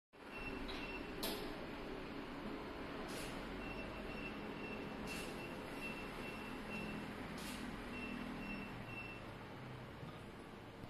Чафин диш MVQ 1011L от ПРАЙД ГРУПП
ЧАФИН ДИШ серии Lux уникален крышкой которая очень плавно закрывается «soft-close».
Стеклянная крышка позволяет гостю не открывая  Чафин Диш, посмотреть блюдо, а клапан препятствует запотеванию стекла.
Сбоку мармита съемный держатель для гарнирных приборов.
В комплекте горелка  для горючего геля или сухого спирта, которую располагаем под основанием Чафин Диш.